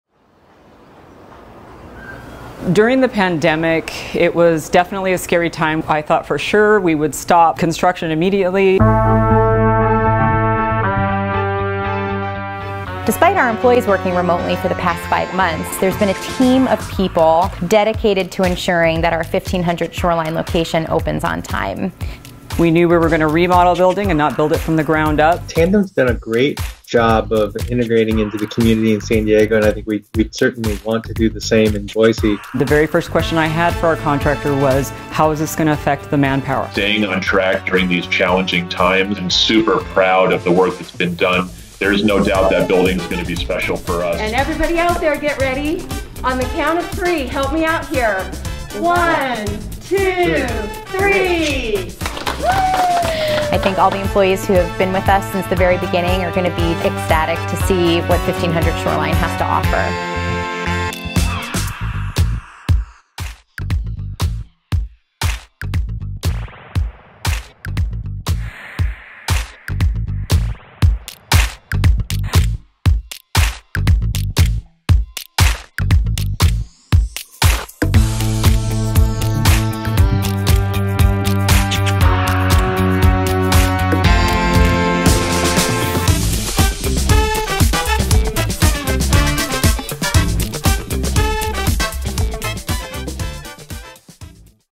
Building Tandem In Boise
It took innovation, creativity and hard work, but we were able to successfully open our new office during a pandemic and we can't wait until we can welcome everyone inside.